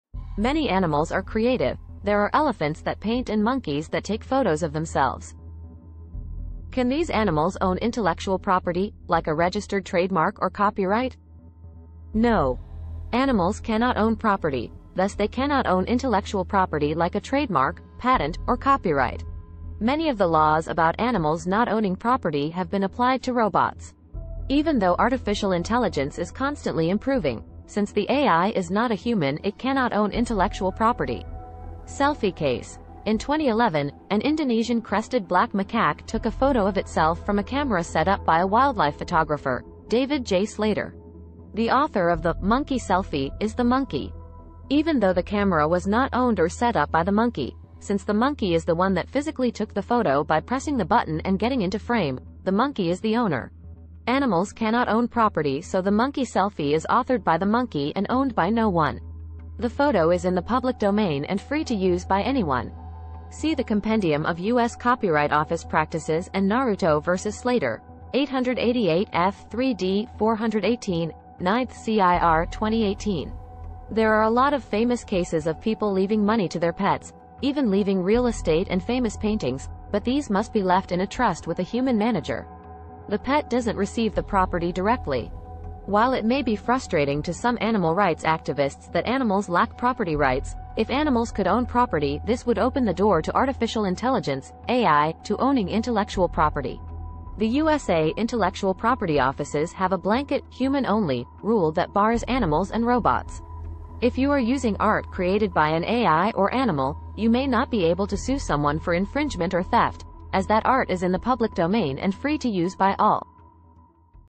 Can animals or robots own a trademark, patent, or copyright?
This video talks about animals and intellectual property protection.